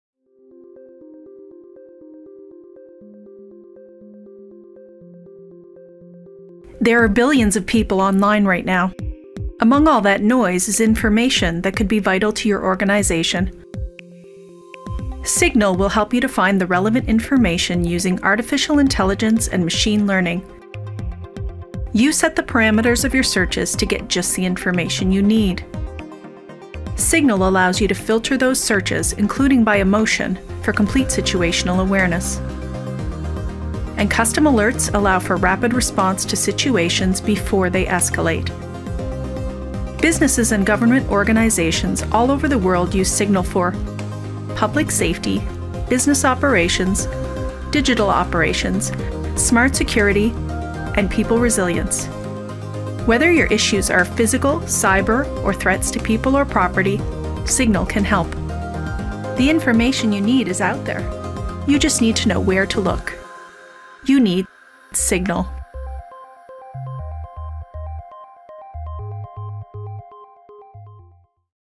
Everbridge Signal - Open Source Threat Intelligence to Keep People Safe and Operations Running
There are billions of people online right now. Among that noise is information that could be vital to your organization’s safety and security. Everbridge Signal will help you find relevant information using Artificial Intelligence and Machine Learning. Detect incidents in real-time by gathering data from public sources including the dark web, deep web and social media. Whether your issues are cyber or physical, Signal can help.